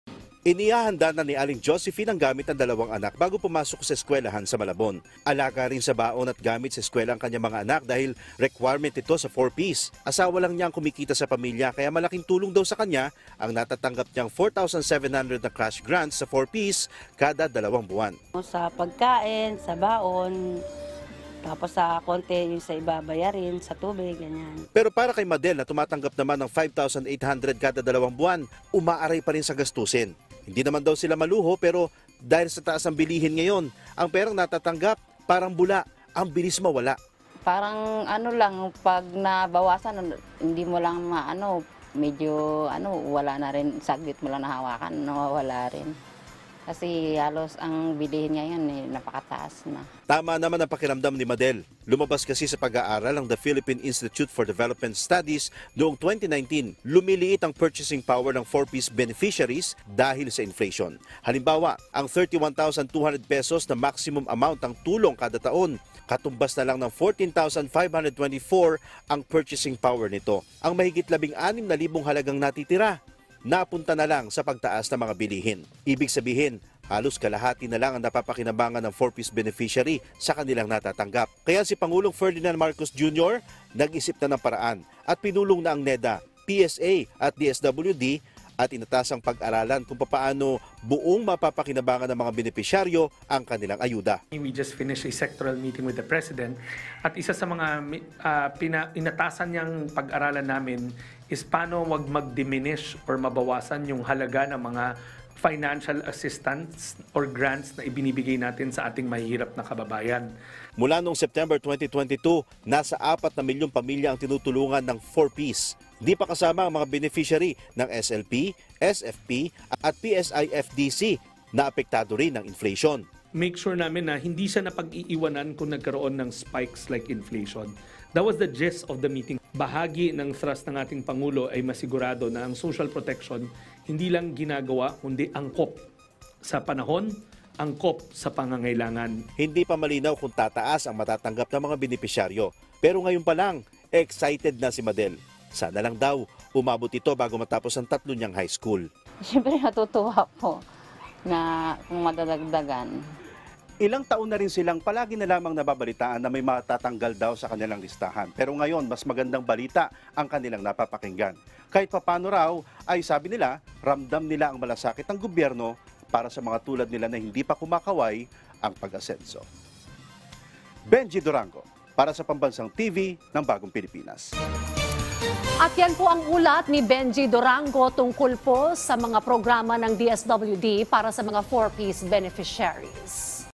Purchasing power ng 4Ps beneficiary, lumiliit dahil sa inflation batay sa isang pag-aaral
PBBM, pinulong ang ilang ahensya para pag-aralan ang isyu sa 4Ps